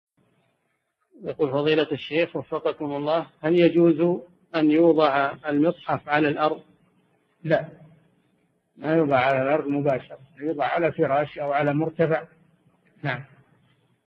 [379 /641] هل يجوز أن يوضع المصحف على الأرض؟ الشيخ صالح الفوزان
[379 /641] السؤال: هل يجوز أن يوضع المصحف على الأرض؟
 الجواب: لا، ما يوضع على الأرض مباشرة، يوضع على فراش أو على مرتفع .
السيرة الذاتية :
للاشتراك في قناة الشيخ صالح الفوزان
أسئلة وأجوبة من شرح كتاب العبودية ( 641 ) مقطع

للاستفسار والإقتراحات والدعم
رئيس الفريق
لمشروع كبار العلماء